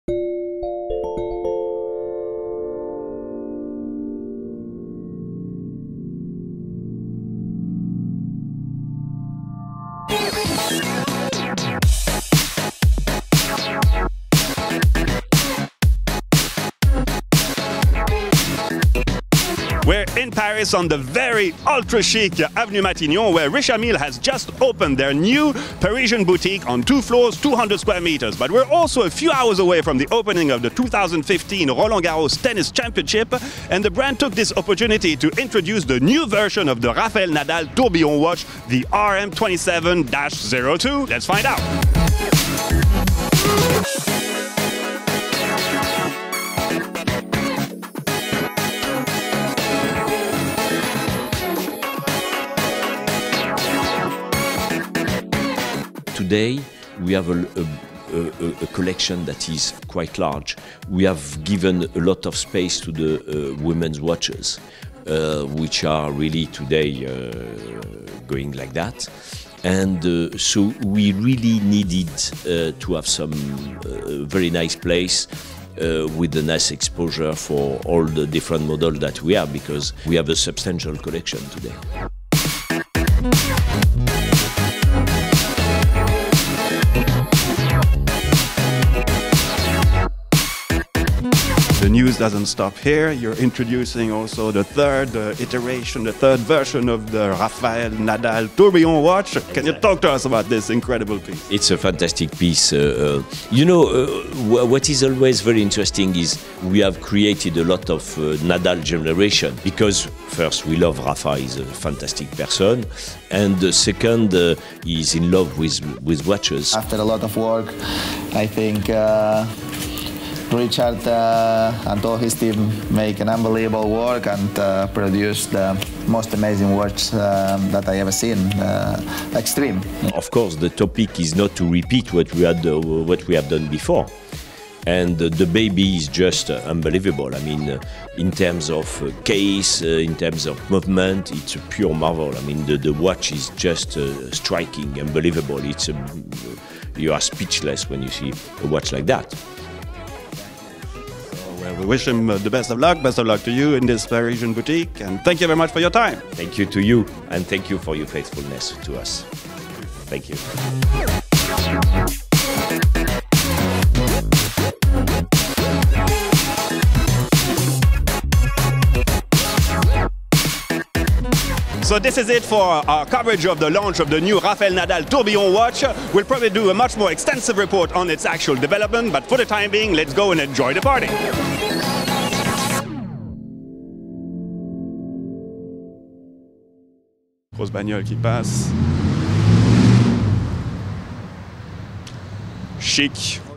Richard Mille Introduces The Extreme RM 27-02 Nadal And Opens A New Boutique In Paris!
The orange strap of the new RM 27-02 “Rafael Nadal” watch by Richard Mille catches the eye. It evokes in fact the clay courts' color of Roland-Garros, the French Tennis Open Mecca. But this third version of what has become the most extreme line at Richard Mille has a lot more to say. Indeed, the top and back of the case are made out of a mix of NTPT carbon and TPT quartz, which is what creates the irregular alternation of white and black colors. The skeletonized movement has arched bridges, the bezel and back are mounted directly onto the unibody baseplate for more rigidity, and the watch boasts 70 hours of power reserve. Surprisingly, the weight of this new version wasn’t released. We guess that it is a slightly heavier than the previous because the glass is here in sapphire crystal rather than Plexiglas. Richard Mille took advantage of the Roland Garros Grand Slam tennis tournament in Paris – where Rafael Nadal has already been the champion nine times – to introduce this watch. At the same time, the brand also inaugurated its new boutique close to the Champs-Elysées in the heart of Paris. As Mille says, it was necessary to have a greater place to show properly all the collections that the brand has now.